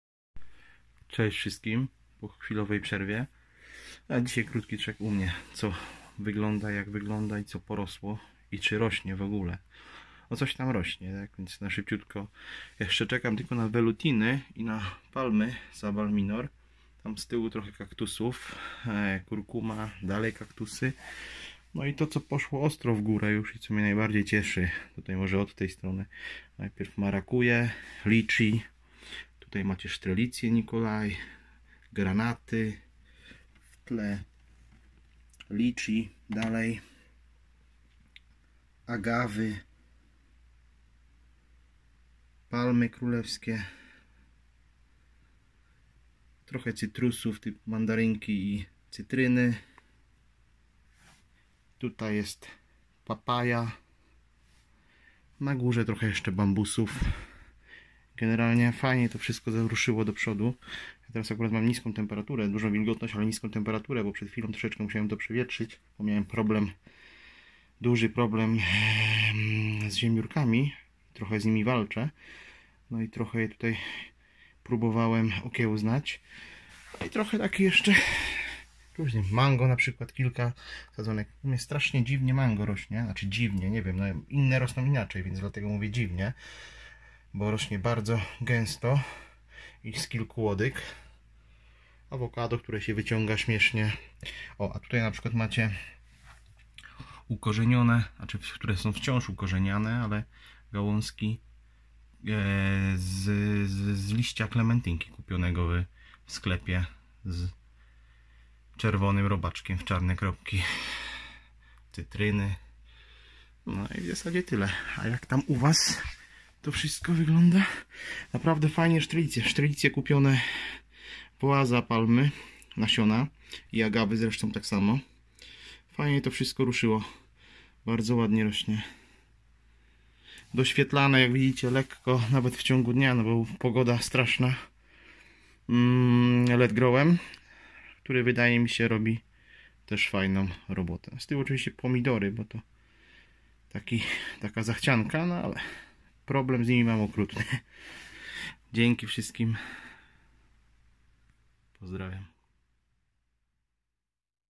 #28 Mango, Awokado, Marakuja, Papaja, Liczi, Granaty, Palma Królewska, Agawa, Strelicia
Co tam urosło w  domowej szklarni ?